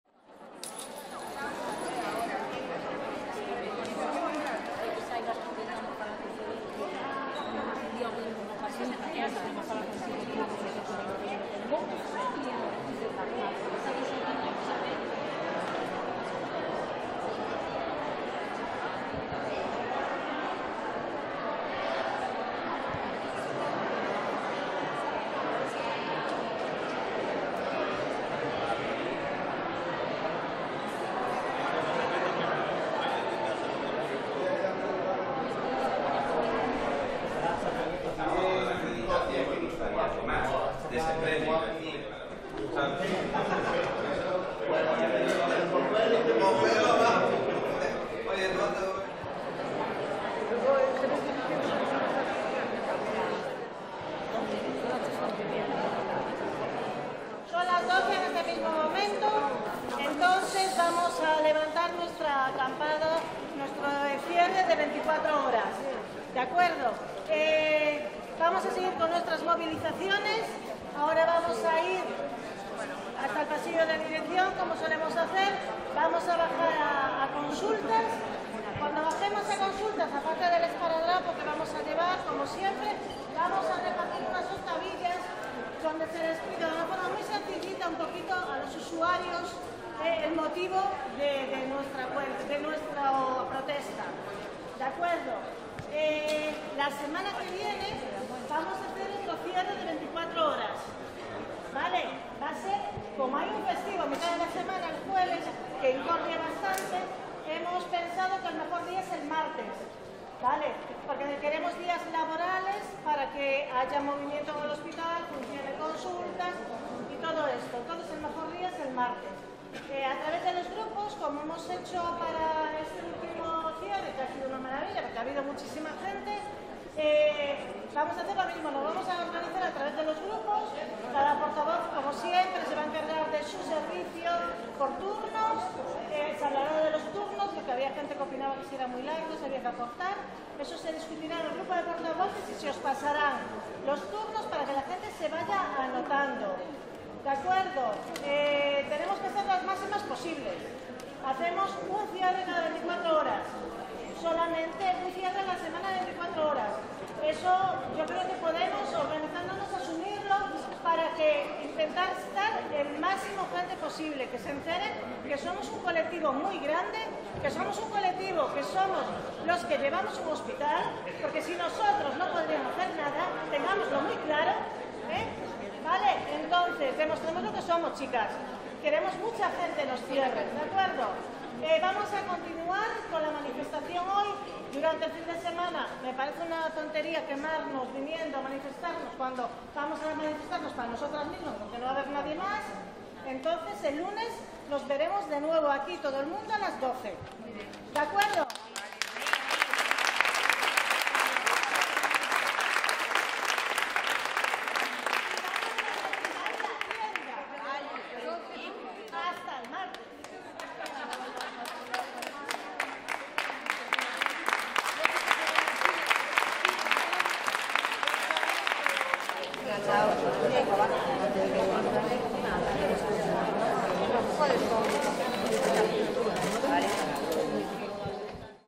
Técnicos Cuidadores Auxiliares de Enfermería, en lucha. 11/05/2018
Los TCAE del Hospital Álvaro Cunqueiro, se encierran en el hall,  para impedir que se les sobrecargue de trabajo con funciones que no le son propias, para que la concesionaria de la recogida de deshechos hospitalarios se ahorre dinero.